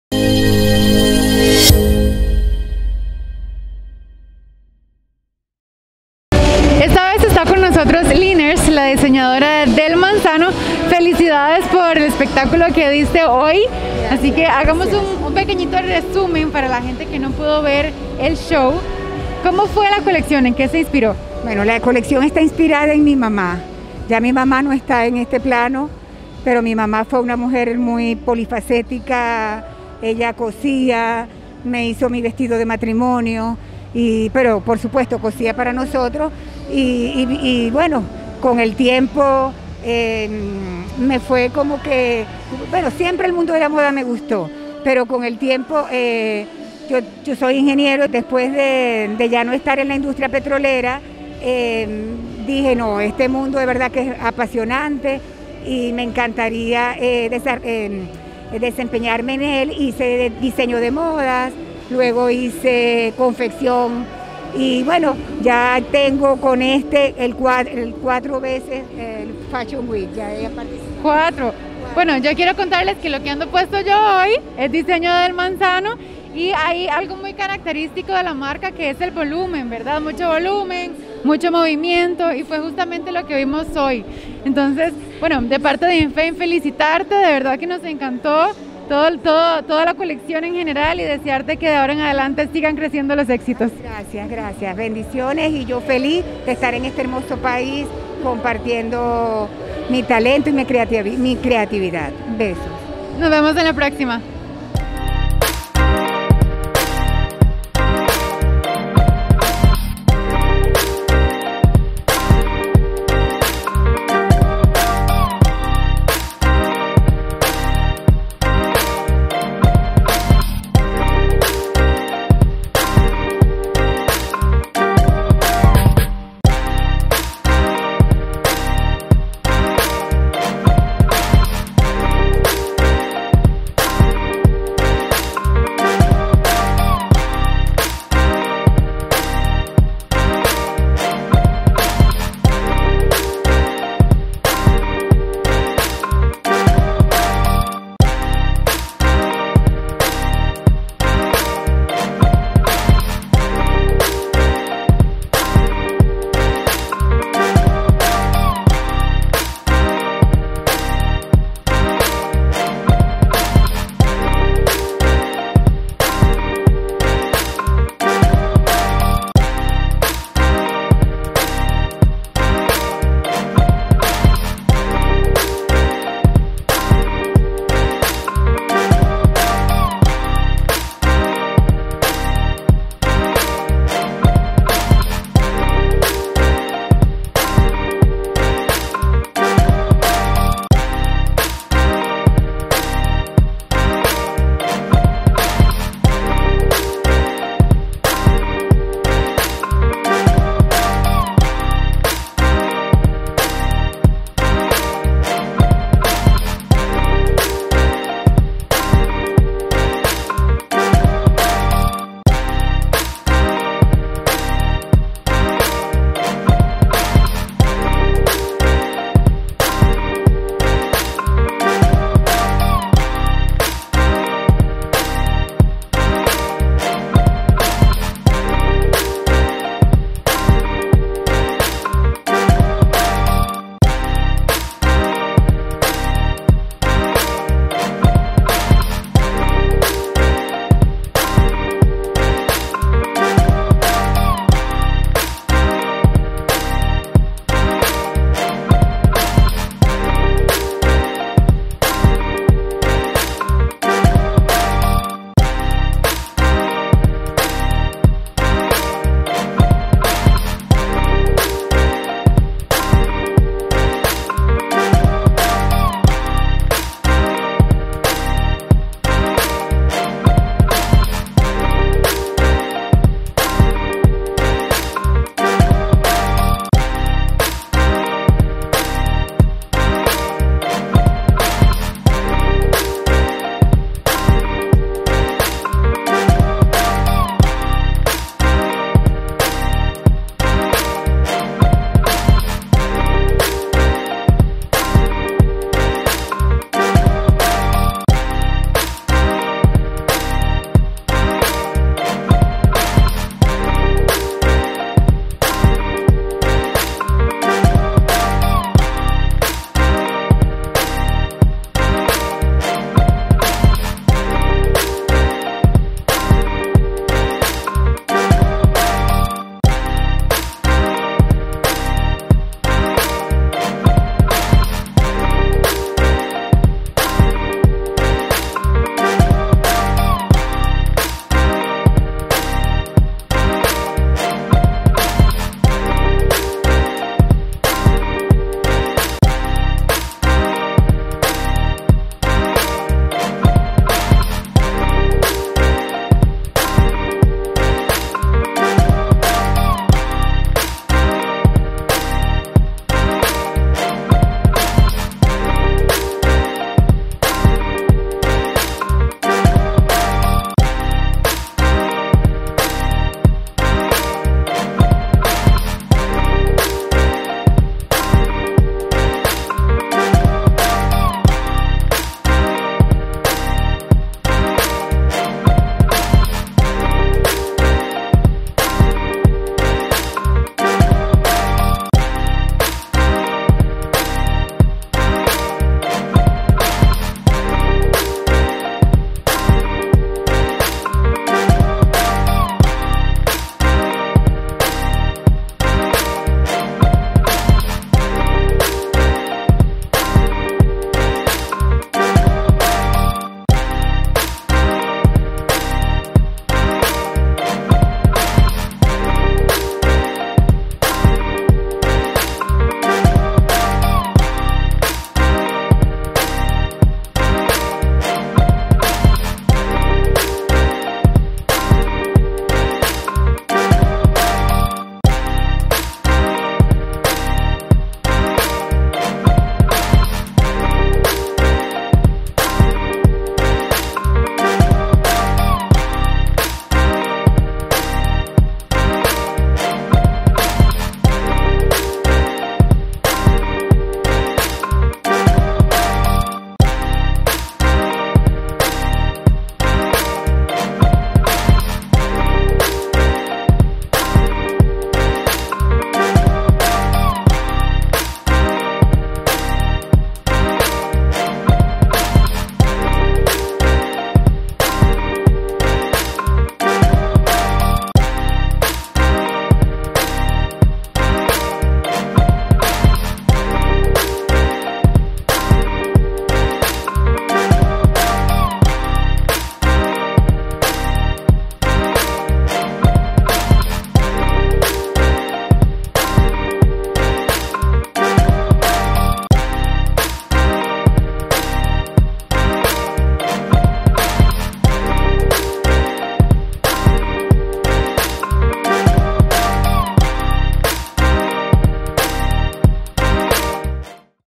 Costa Rica Fashion Week PRESENTA: Del Manzano
De ingeniera a diseñadora de modas, conozca la colección de Lirners en esta pasarela tan especial.